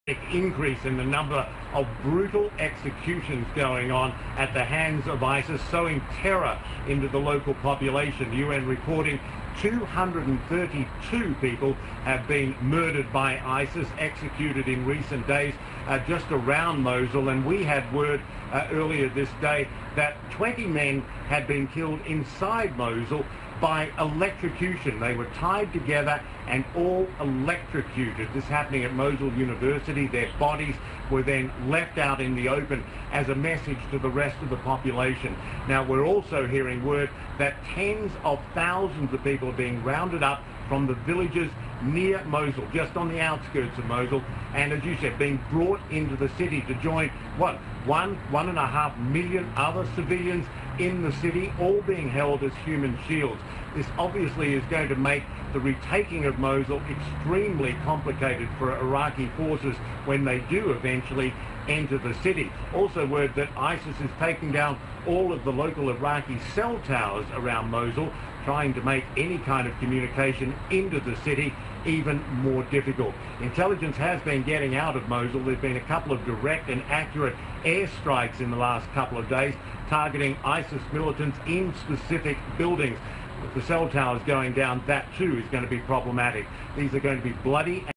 [BREAKING NEWS]ISIS executes 232 near Mosul
ISIS has executed 232 people near the Iraqi city of Mosul and taken tens of thousands of people to use as human shields against advancing Iraqi forces, the United Nations says.